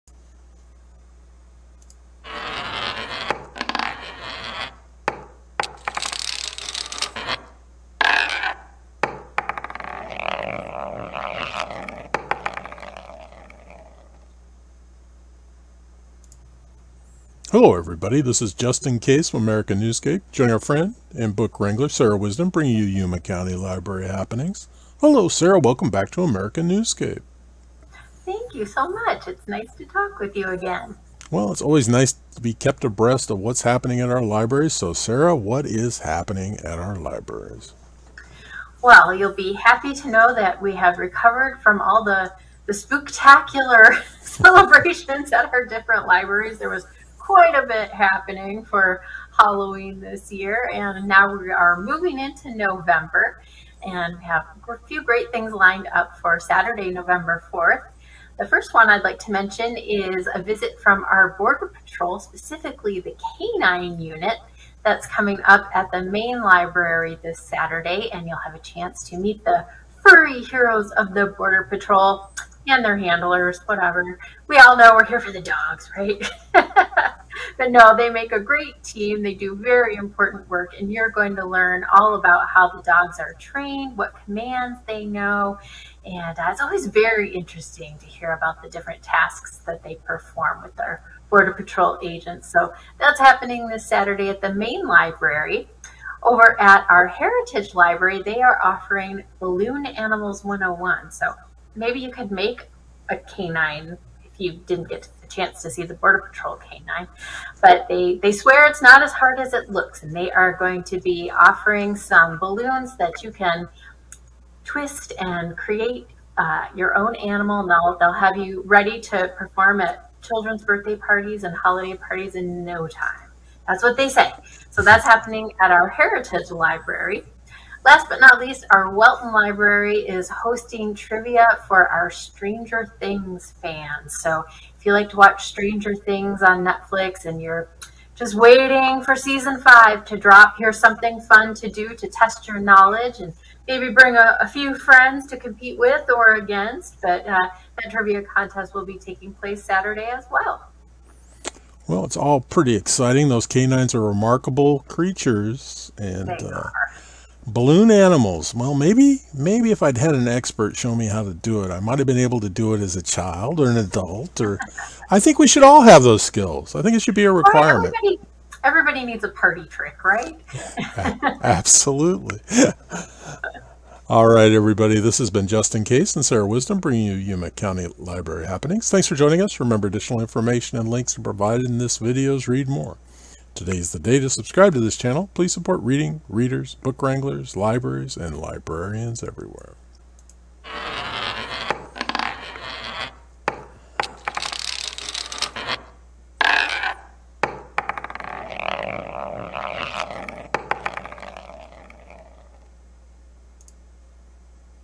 Yuma County Library Happenings with Sarah Wisdom November 13th, 2023
Join Justin Case of America Newscape as he presents and interviews Yuma County Library Happenings with Book Wrangler Sarah Wisdom November 13th, 2023

ZOOM for the Holidays
Wednesday, November 15th @ 1PM
Attendees will create a Zoom account on their own computer or tablet, and learn how to use the software in time to visit with friends and relatives who are far away for the holidays. Space is limited. The Main Library is located at 2951 S 21st Drive.

Nature Scavenger Hunt
Saturday, November 18th @ 1PM
Let’s go on a nature hunt and create a nature print. This program is for young botanists ages 6–12 with a caregiver. The Foothills Library is located at 13226 E South Frontage Road.

Backyard Citrus Growing
Saturday, November 18th @ 2PM
From selecting, planting, and pruning techniques, discover the secrets of nurturing vibrant citrus trees and savor the sweet rewards of your own backyard citrus orchard. Space is limited. The Heritage Library is    located at 350 S Third Avenue.

America Newscape is a simple resource for all things America through this portal. These productions are not created or meant for children.